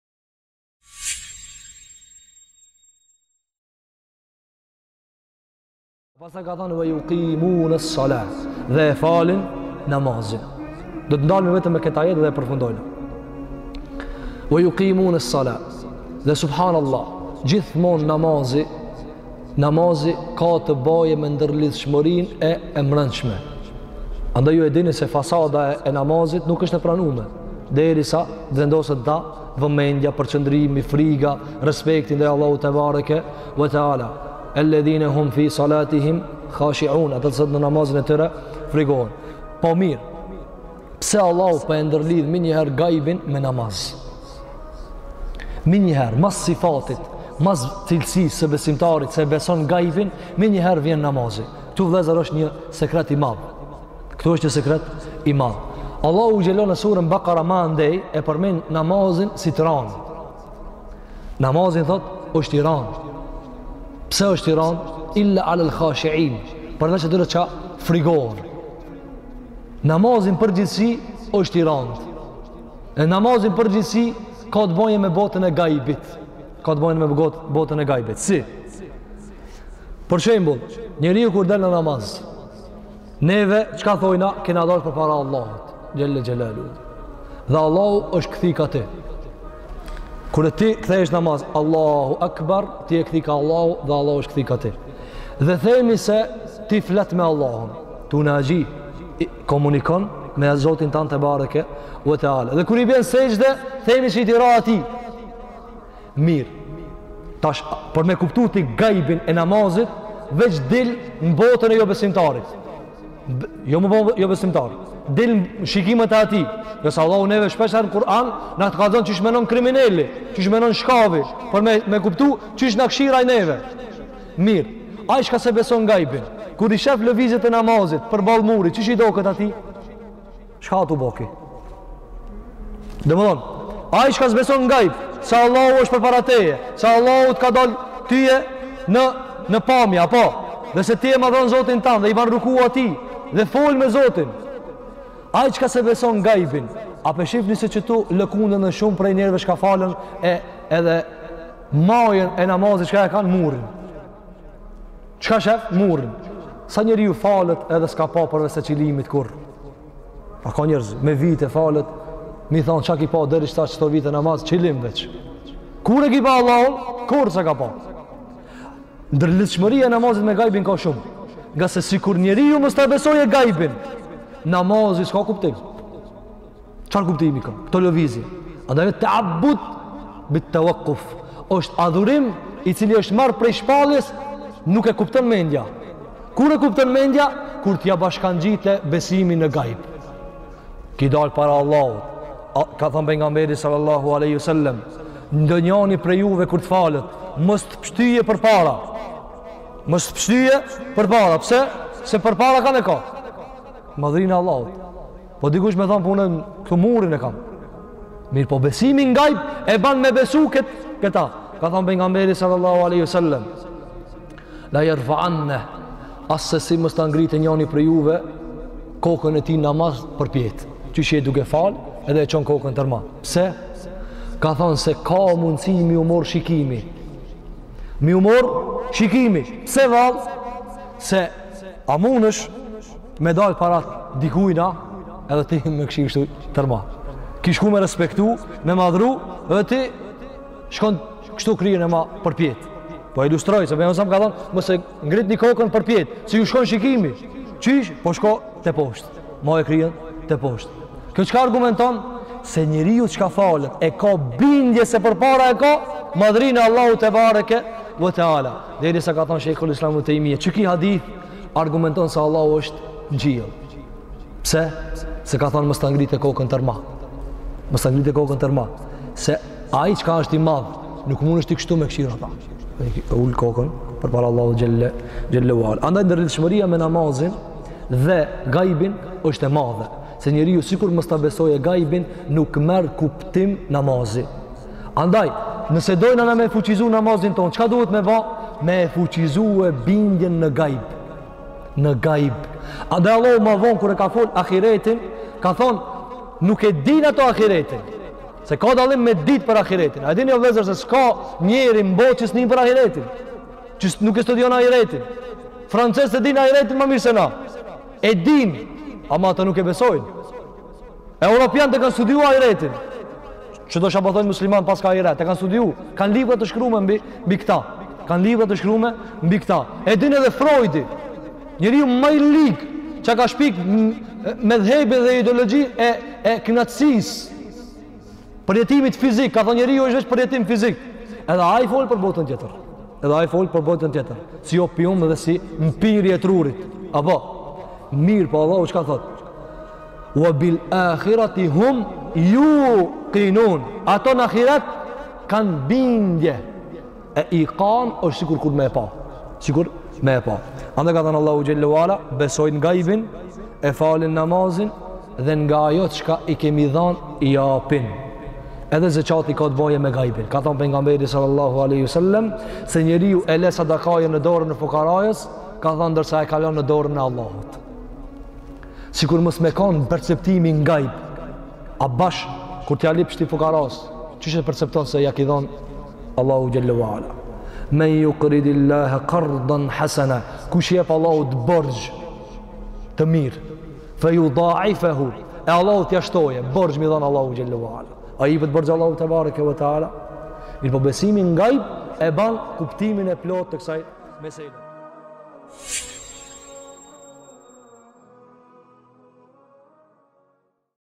Lidhja e namazit me botën e fshehur | MADHËSHTORE
Motivi ju prezanton sekuencën e shkurtë me titull: "Lidhja e namazit me botën e fshehur" nga Hoxhë Qëndrim Jashari.